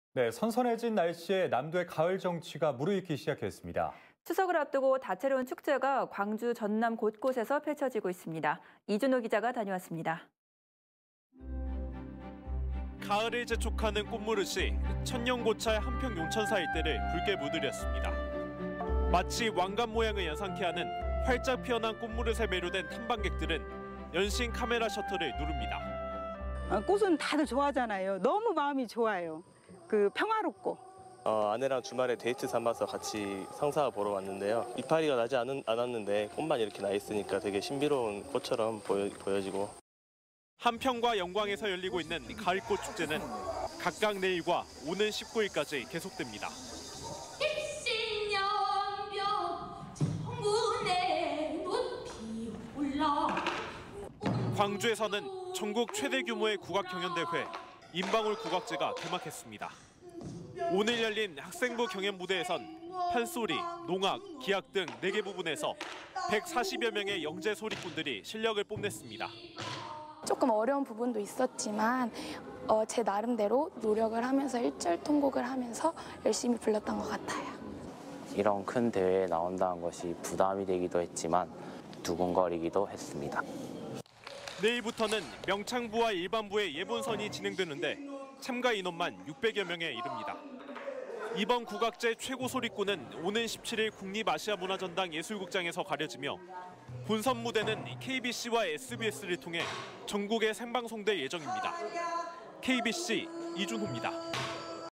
꽃무릇ㆍ국악..깊어가는 가을 남도는 '축제 중'
선선해진 날씨에 남도의 가을 정취가 무르익기 시작했는데요.

추석을 앞두고 다채로운 축제가 광주와 전남 곳곳에서 펼쳐지고 있습니다.

이준호 기자가 다녀왔습니다.